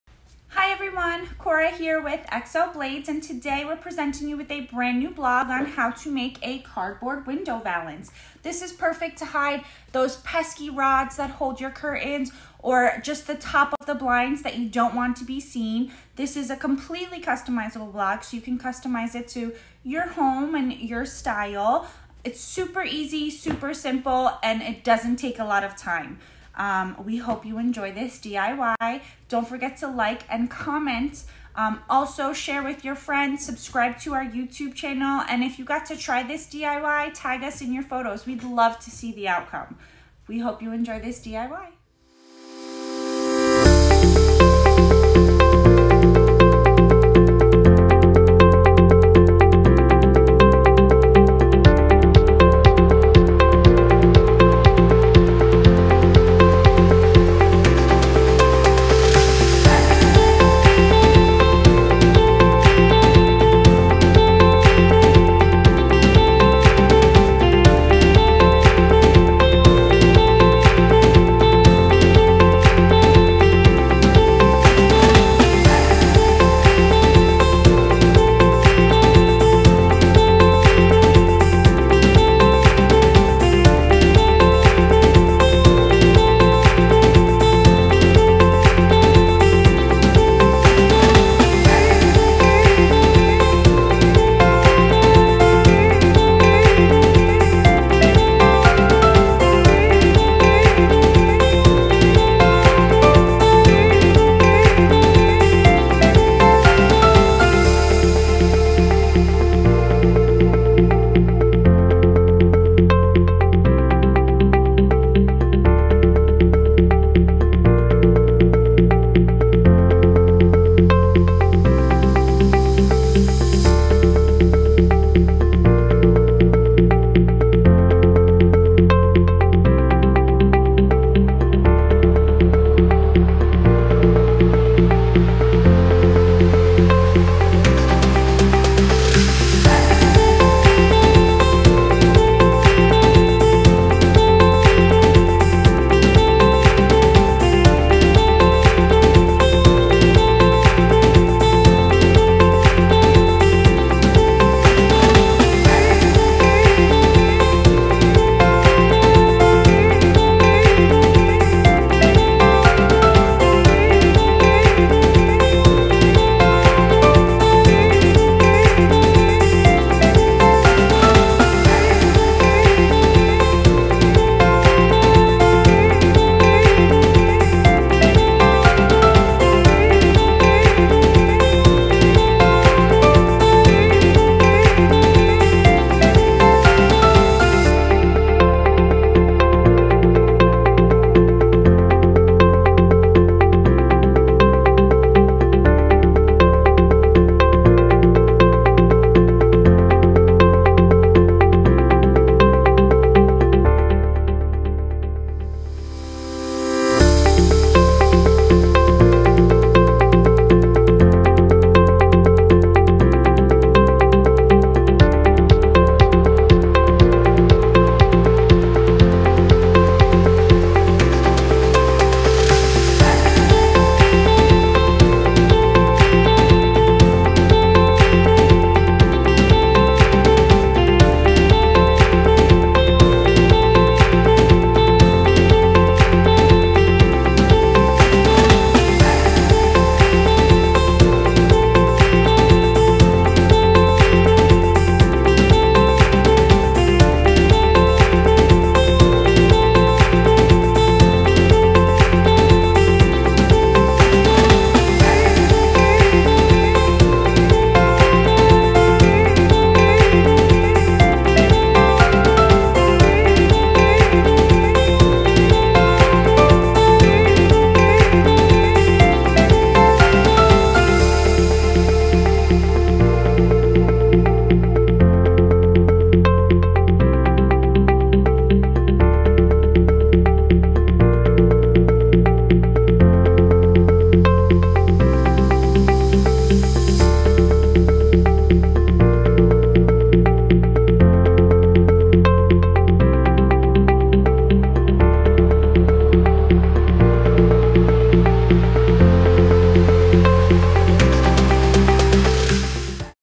How To Make A Cardboard Window Valance - Excel Blades
We love to re-use materials that would typically be thrown out to create easy home décor.  What better way to use that cardboard box you are about to throw out than by creating a window cornice or valance!  No need to spend money on a custom made window valance when you can create it yourself with items you already have around the house.  Instead of using wood, we decided to use cardboard.  It is a lightweight and inexpensive way to add some pizzaz to your home.

Making the window cornice only requires cardboard, fabric, a measuring tape, hot glue, chalk and a hobby knife. Not only is this easy to make, it is easy to install and can be changed at any time.  Make a new window valance for each holiday or keep the same one up all year round.  The possibilities are endless when making this window cornice.  The hardest part about recreating this blog will be determining what color fabric you want to use.

Grab your measuring tape, some coffee and let’s get started!

Materials
Excel Blades K26 Fit Grip Knife – 16027
Excel Blades 16ft Measuring Tape – 70070
Excel Blades 18” x 24” Self Healing Cutting Mat – 60032
Alumicutter Ruler from Excel Blades Alumikit – 90124
Cardboard – A large piece depending on your window size.
Fabric
Hot Glue
Chalk

Step 1: Measure the width of your window from the outside edges of the trim. Measure the depth the cornice board will need to be from the wall past the rod/blinds.  Determine the height you want the valance to be.
Tip: When measuring the depth, make sure you add an inch to your total measurement to ensure the valance is not only flush against the wall, but to make sure that it will not hit the rod/blinds.

Step 2: Once you have taken all your measurements, grab your cardboard.  Measure each piece of the valance individually.  Our window was 25” wide and we wanted the valance to be 5 inches tall.  We drew a rectangle with the chalk on the cardboard measuring 25” x 5”.  Then, we cut our rectangle with our K26 Fit Grip Knife.
Tip: Be sure to measure twice and cut once.  You always want to make sure that your measurements are correct before you start cutting.

Step 3: Next, measure and draw the sides of your valance.  Our sides were 3” wide and 5” high.  Once you have those measurements drawn out on your cardboard, cut them out.

Step 4: Then, measure and draw the top of your valance.  The top of our valance is 25” x 3”.  Once you have those measurements drawn out on your cardboard, cut them out.

Now, let’s glue the valance together!

Step 5: Grab your hot glue gun and cardboard pieces.  Gluing one side at a time, add glue to the edge of your cardboard.  Then, apply it to the respective cardboard piece it should be attached too.  Continue this process until your valance is glued together.  Once all your pieces are together, let the valance dry.

Step 6:  While the valance is drying, grab your fabric and measuring tape again.  Measure the cardboard from one end to the other including the sides of the valance.  This is to ensure the fabric will wrap completely around and the valance and there will be no sign of cardboard.  Add 2 inches to your final measurement.  Repeat this process for the front and top of the valance.  Add 2 inches to your final measurements for each as well.

Step 7: Measure your fabric, then with your chalk draw the size you need for your valance. Using your K26 and Alumicutter ruler, cut the fabric.
Tip: Using your ruler while cutting the fabric will help guide you in a straight line.

Step 8: Grab your glue gun, fabric, and cardboard valance.  Start to glue the fabric to your cardboard. Make sure you leave extra fabric on the ends to wrap around the edges, this is to ensure that the cardboard will not show.

Step 9: Once you have glued everything in place.  Turn your valance over and glue the edges to the inside of the cardboard so they are not visible. Once it is dried you will have completed your window valance!

We loved the way this window valance turned out!  Not only is this a quick DIY, it’s easy and can be customized to fit your style.

We hope you enjoyed making this valance as much as we did!

Let us know what you think by leaving a comment!